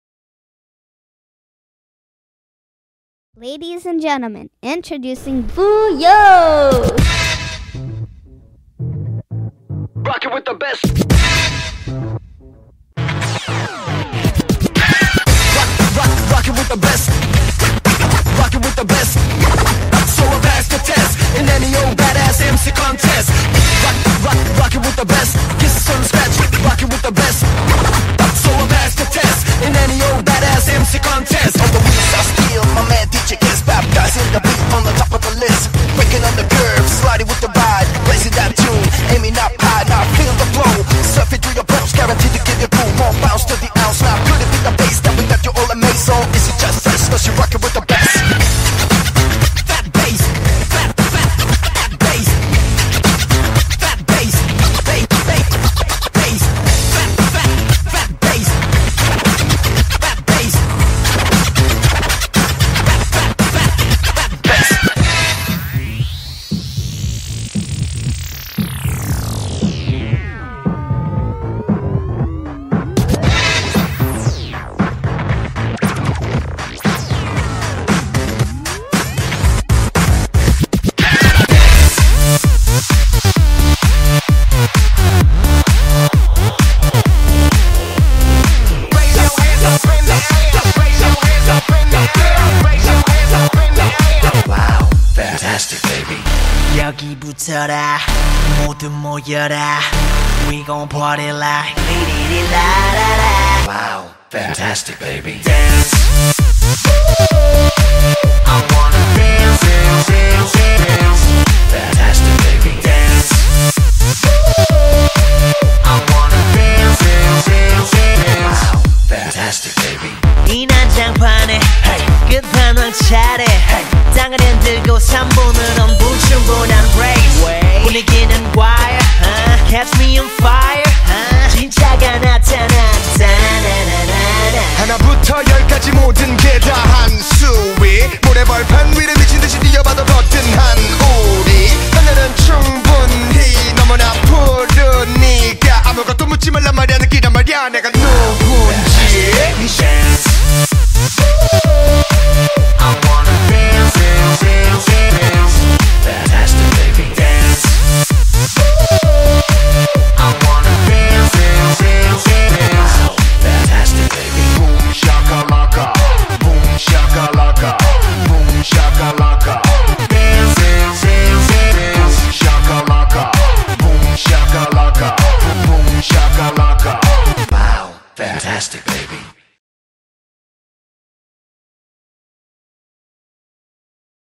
FUYO Crew @ Swaggout 4
Fuyo! One of Singapore's best B-Boy crew tearing the stage with the amazing moves!